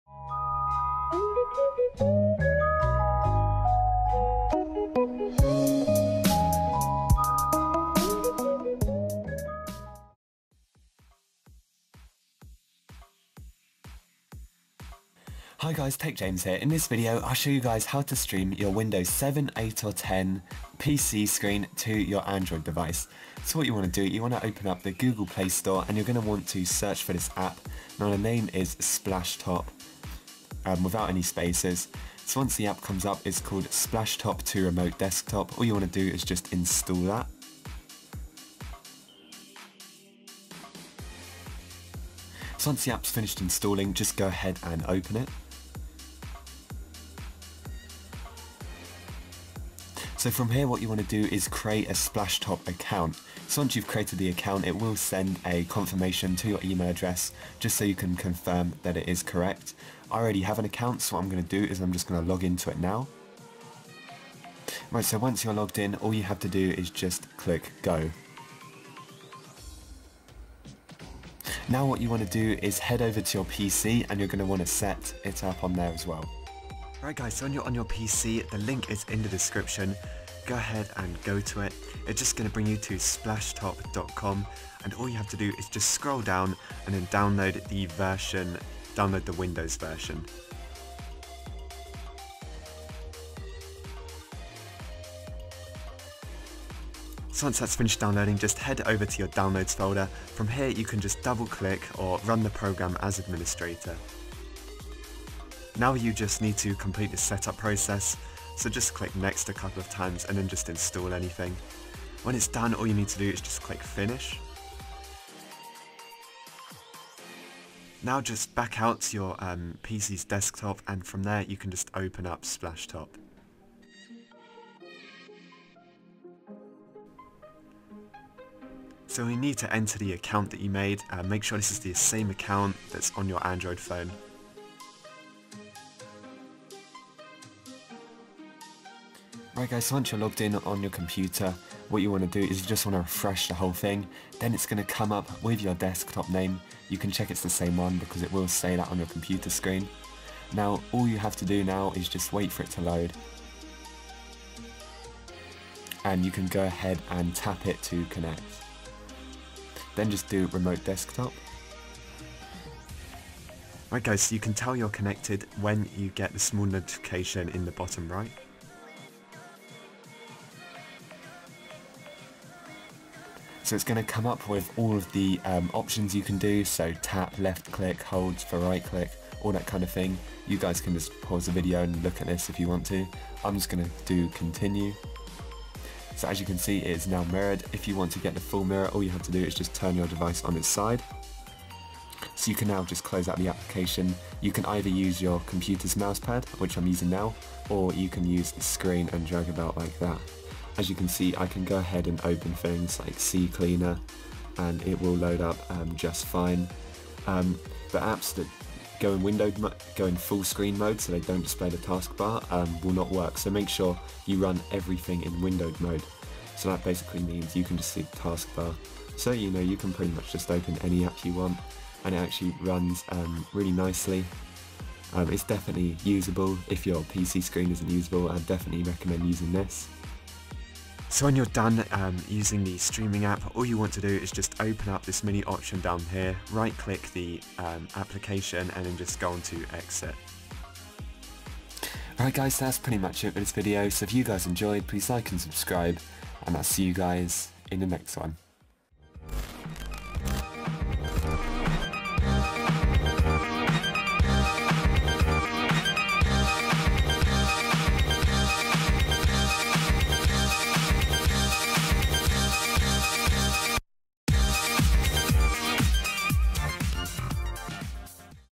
Stream Your Windows PC Screen To Android! (Splashtop)
For this video, I’ll show you guys how to stream your Windows 7/8/10 PC screen to any Android device!